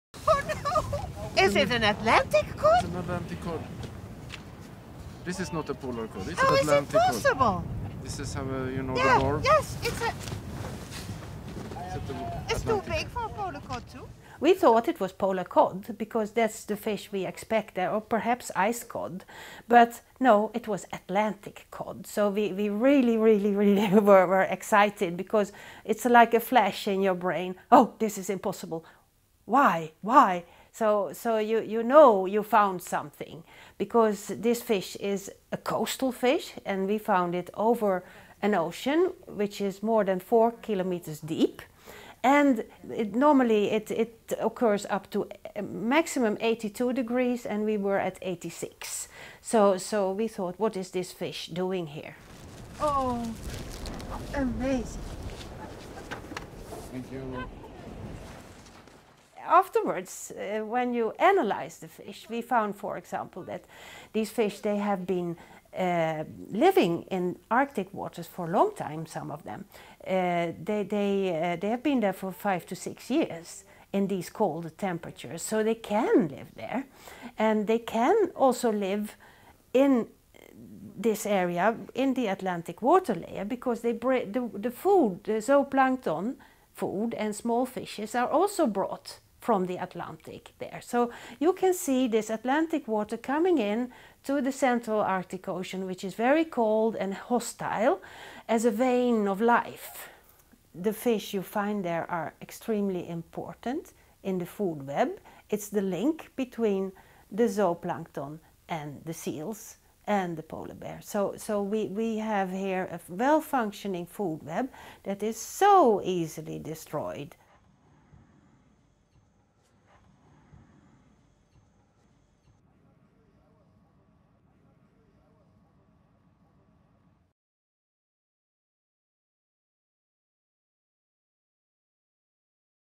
Unexpected Fish and Squid Found in the Central Arctic Ocean
Single individuals of Atlantic cod and squid occur much further north than previously expected. Scientists participating in the international MOSAiC expedition with research icebreaker Polarstern have found fish and squid in deep water in the middle of the Arctic Ocean. The results from Stockholm University, the Alfred Wegener Institute and colleagues in the European Fisheries Inventory in the Central Arctic Ocean (EFICA) Consortium are published in the scientific journal Science Advances.

Credit: Stockholm University